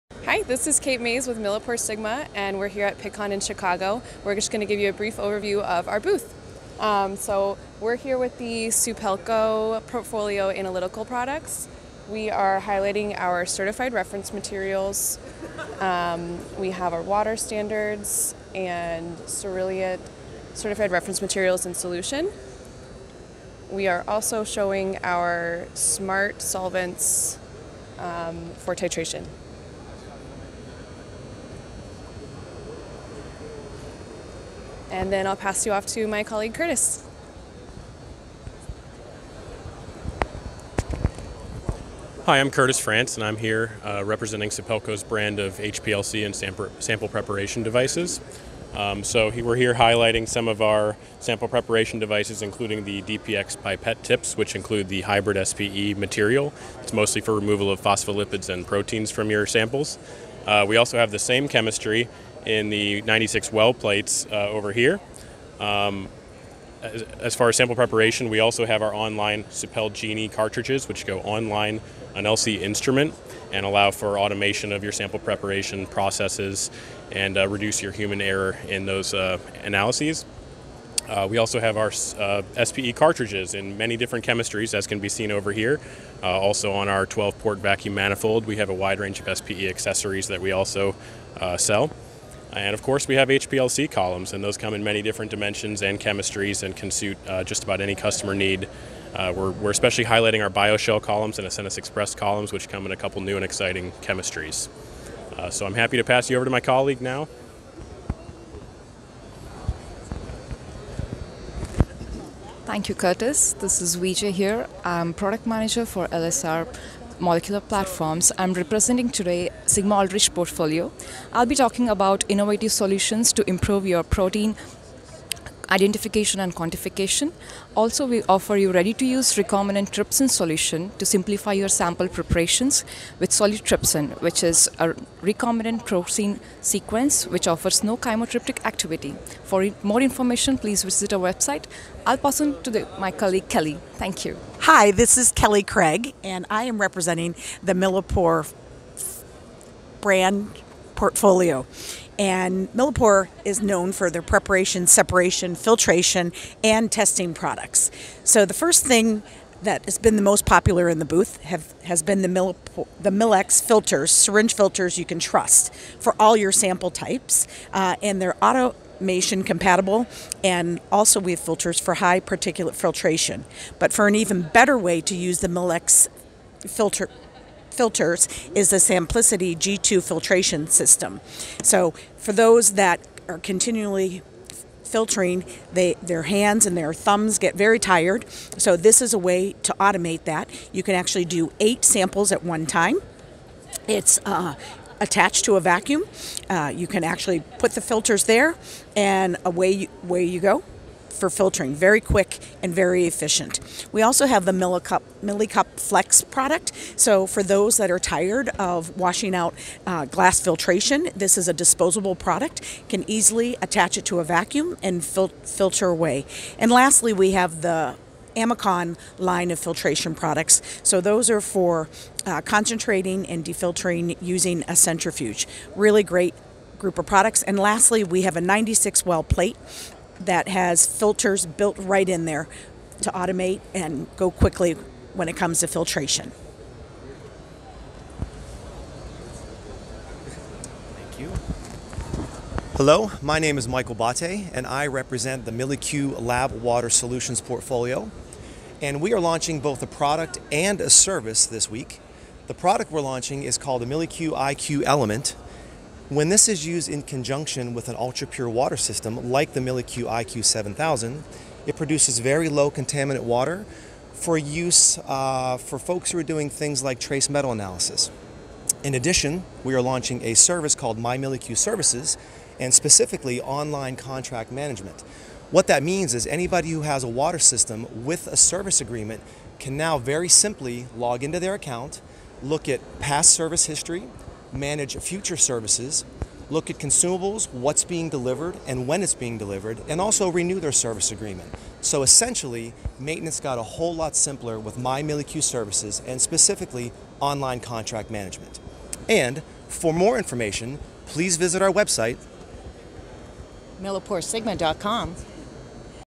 LiveWithChaudhrey at Pittcon2020 Chicago with MilliporeSigma
Here's a video tour I did from the MilliporeSigma booth at Pittcon last year where
Kate Mayes from the Supelco portfolio spoke about their range of certified reference materials and smart solvents.
Curtis Frantz from the Supelco portfolio went through their SPE and HPLC solutions
Vijaylakshmi Kulkarni from the Sigma-Aldrich Portfolio discussed their Solu-Teypsin and SILu- Mab innovative tools to improve protein identification and quantification,
@Kelly Craig showcased the Millipore portfolio sample oreo and filtration solitions and,
Michael Botte from the @Milli-Q Lab Water portfolio talking about their new Milli-Q IQ Element and their new My Milli-Q Services.